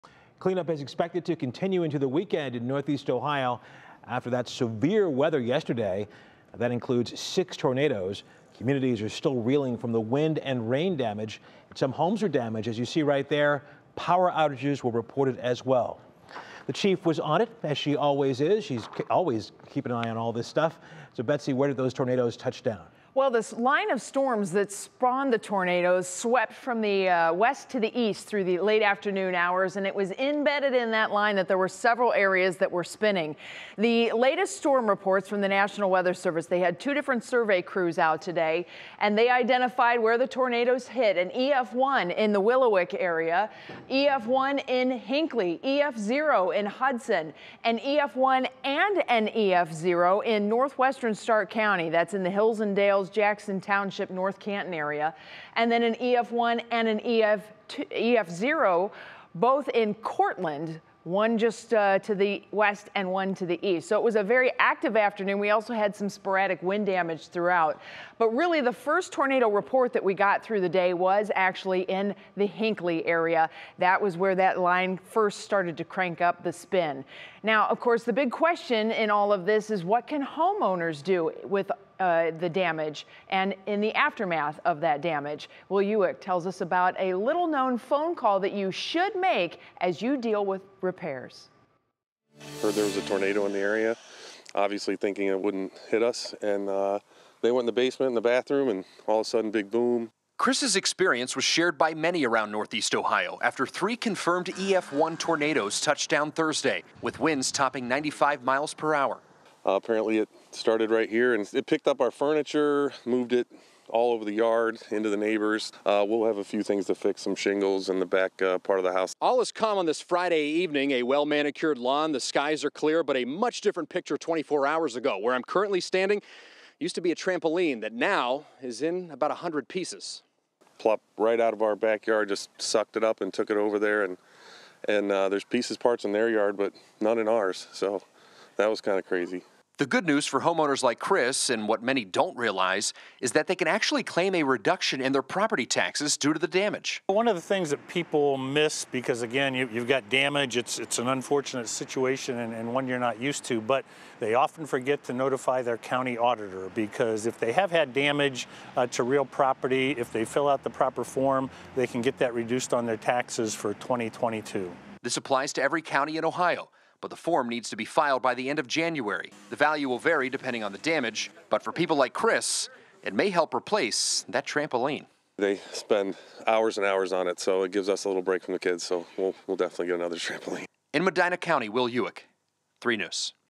A little-used resource for homeowners with storm damage
After tornadoes popped up throughout Northeast Ohio, homeowners who suffered damage are wondering what all they should do.

However, there is one thing that many forget about that could save damaged property owners some money. What many don't realize is they can claim a reduction in their property taxes due to the damage.

"One of the things that people miss — because again you have damage and it's an unfortunate situation and one you’re not used to — but they often forget to notify their county auditor," Mike Kovack, who holds the position in Medina County, said. "If they have had damage to real property, if they fill out the proper form, they can get that reduced on their taxes for 2022."